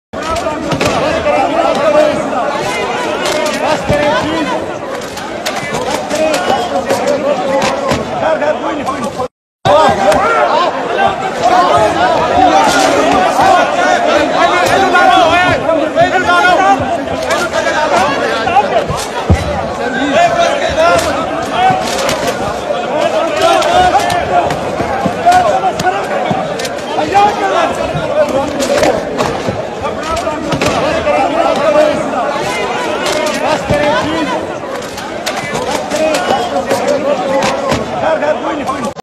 Fighting inside pakistan parliament
Politicians are fighting among each other inside pakistan parliament.